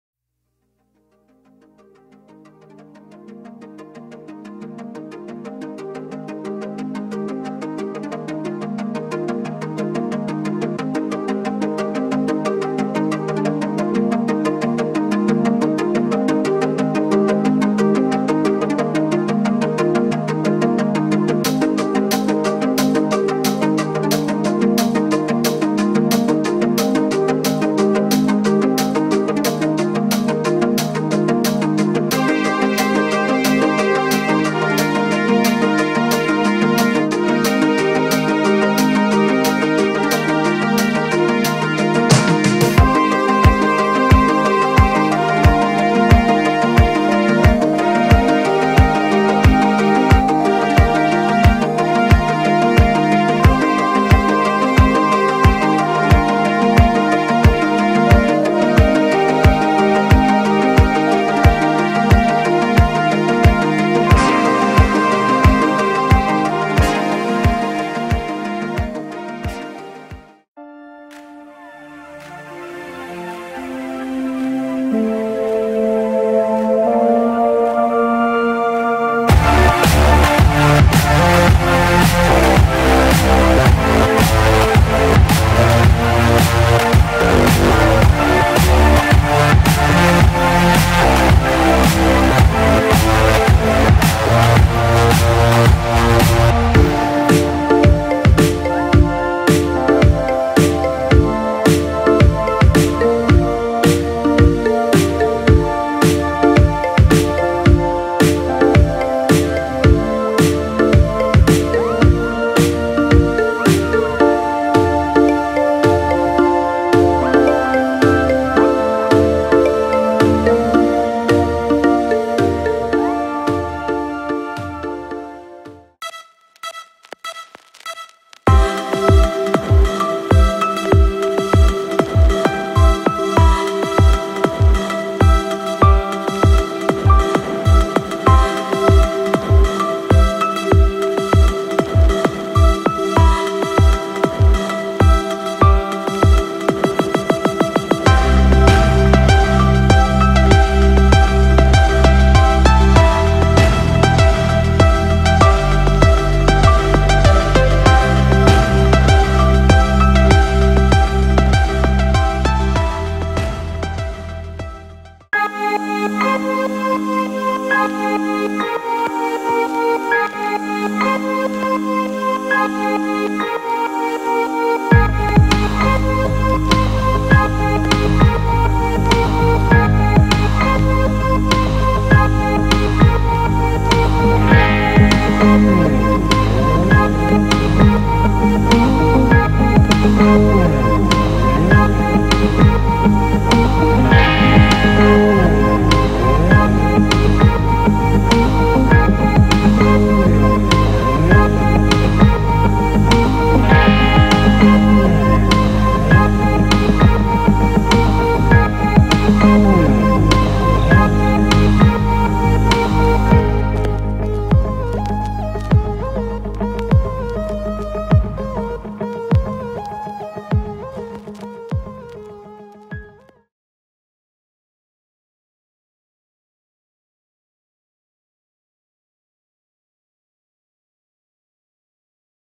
Barcelona street artists black book drawings part 1
Barcelona based street artists stop motion animations reaturing Seclestyle, Rombillos, Konair and Arububu. All filmed on location in Barcelona as part of mybarcelona